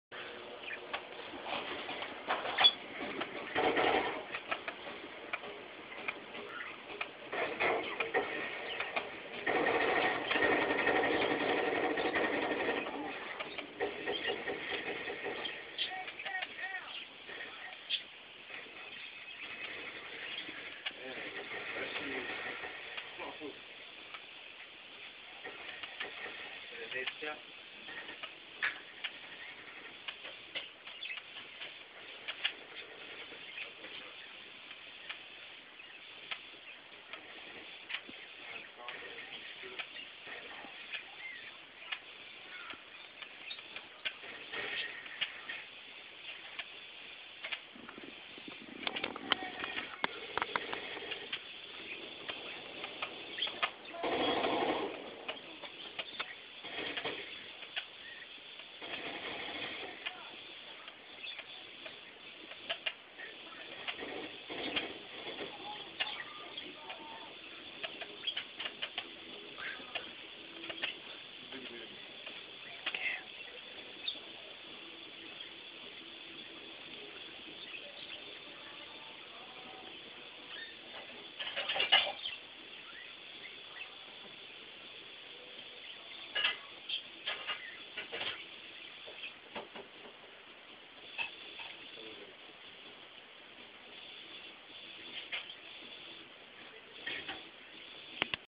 My cockatoo does the nasty
My cockatoo doin' the nasty. Her previous owner never paid attention to her so she learned to entertain herself. Can't get her to talk yet because she seems preoccupied. LOL